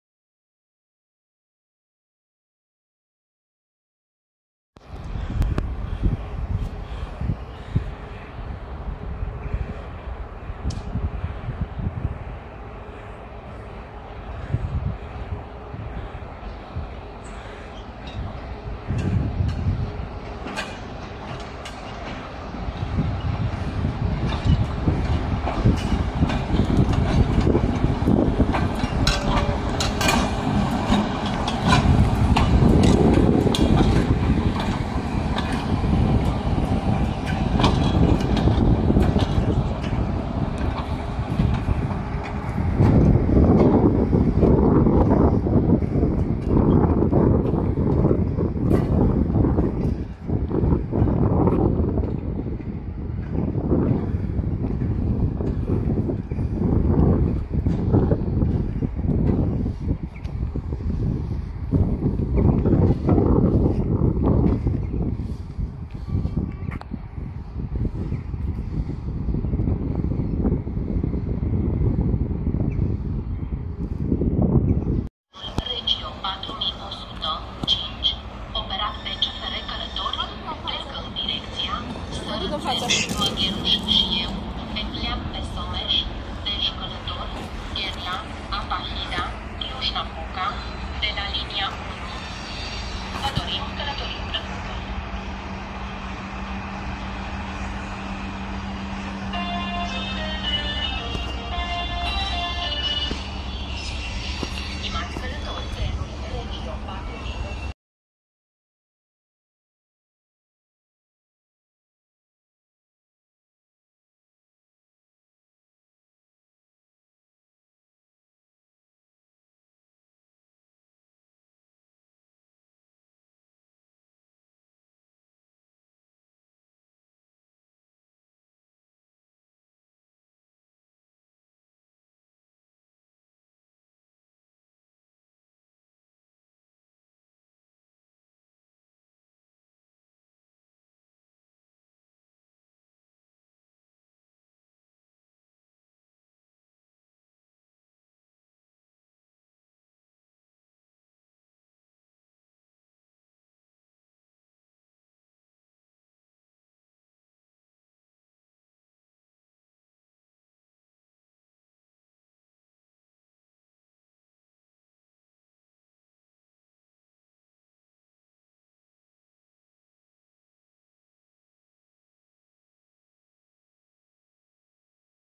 Manevre 060-EA si LDH (CFR Marfa) in Gara Bistrita Nord [Septembrie 2020]
00:05 - Manevra EA 025 apartinand CFR Marfa

01:15 - Manevra LDH 003 al CFR Marfa

01:48 - Manevra EA 025 apartinand CFR Marfa